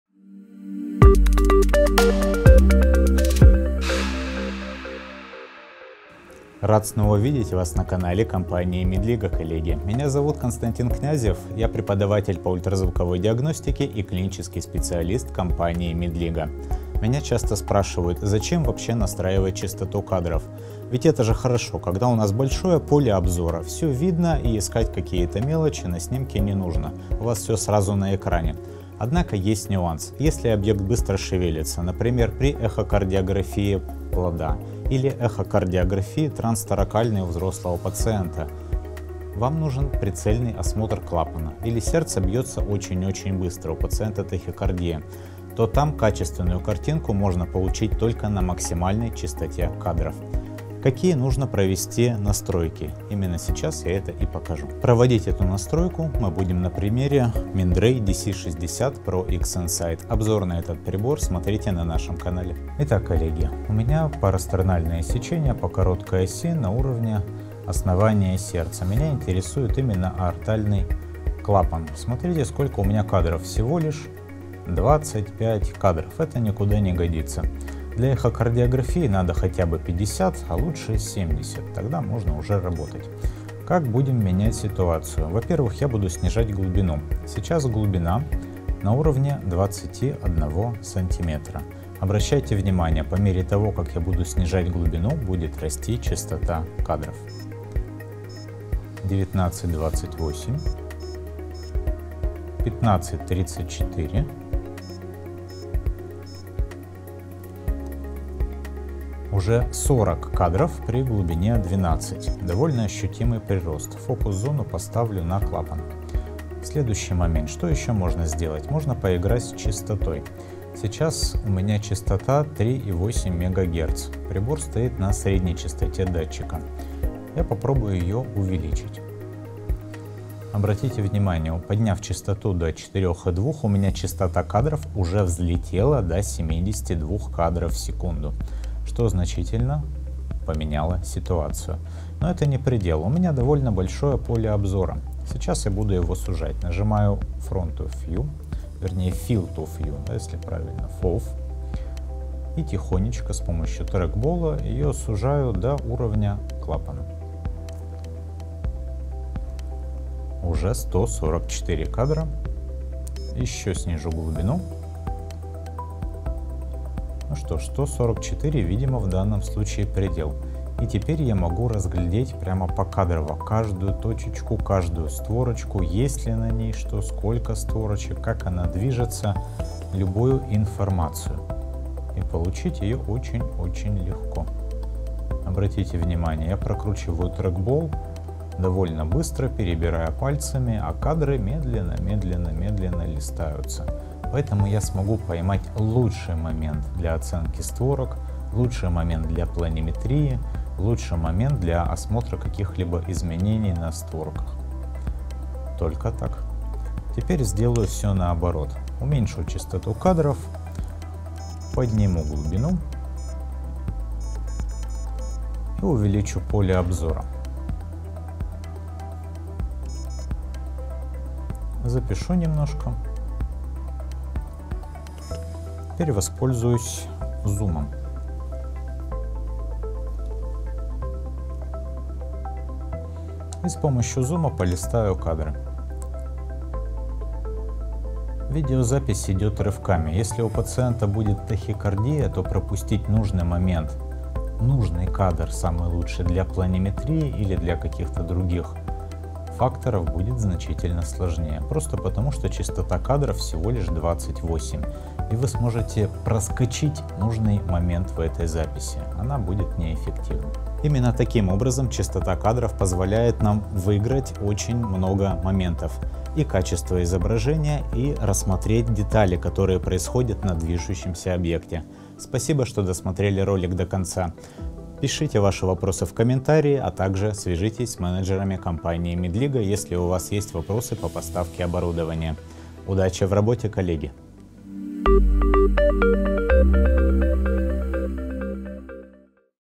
Настройка частоты кадров на примере Mindray DC-60 Pro
В каких случаях необходимо увеличить или уменьшить частоту кадров на ультразвуковом аппарате? Как влияет частота кадров на качество картинки при эхокардиографических исследованиях (ЭхоКГ)?

В новом мастер-классе компании MEDLIGA, вместе с Константином Князевым - клиническим специалистом, преподавателем и автором курсов для врачей ультразвуковой диагностики, расскажем, в каких случаях необходимо настраивать частоту кадров и как она влияет на качество изображения.

В практической части мастер-класса доктор на примере УЗИ аппарата Mindray DC-60 Pro проведет настройку прибора, необходимую для увеличения или уменьшения частоты кадров, а также сравнит полученные изображения.

По вопросам медицинского оборудования пишите нам и звоните по тел.: , поможем с выбором необходимой медицинской техники под ваши задачи и бюджет.

00:06 - Вступление.
00:28 - В каких случаях необходимо настраивать частоту кадров?
00:50 - Настройка увеличения частоты кадров.
03:26 - Уменьшение частоты кадров в эхокардиографии.
04:21 - Заключение.